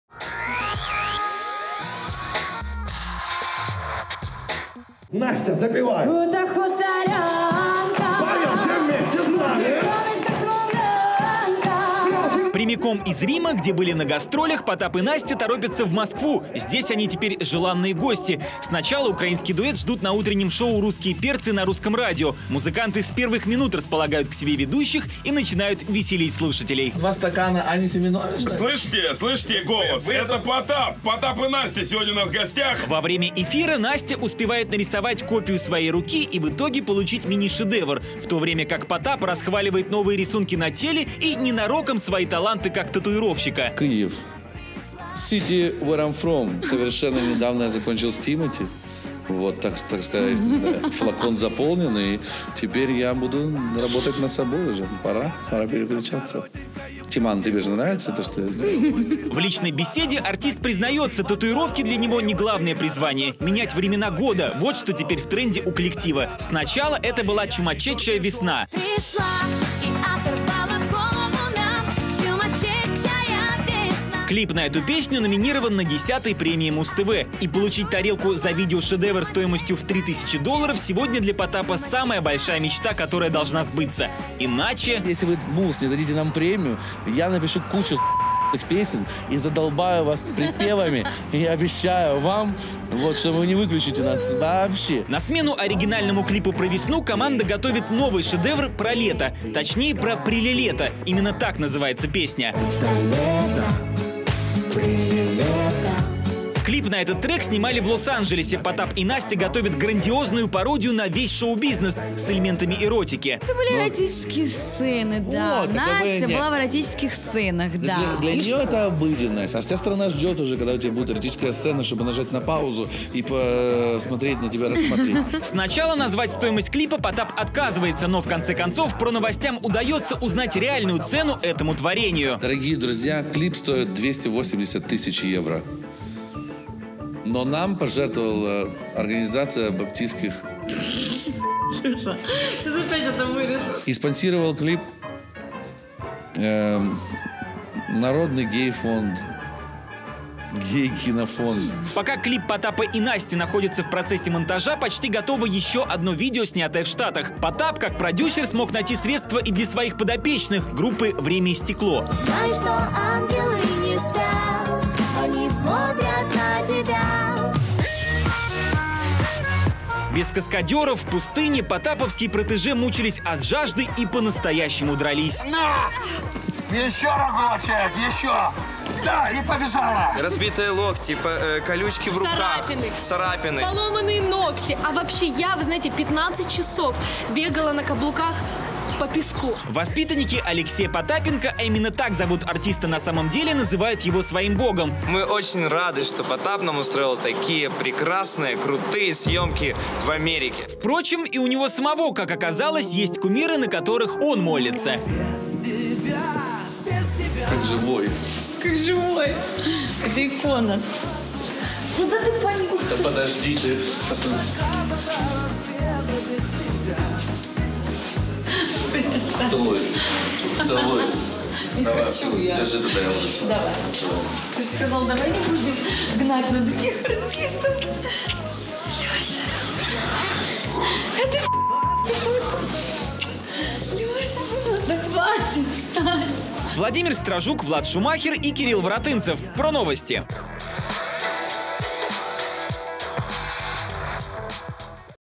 Потап и Настя целуют лик блаженного Стаса Михайлова
Смотрите видео как @potup и @KAMEHCKUX показали нам татуировки и облобызали лик блаженного Стаса Михайлова! А также - группа Время и Стекло: мучения в американской пустыне.

Автор - Владимир Строжук
Операторы - Влад Шумахер и Кирилл Воротынцев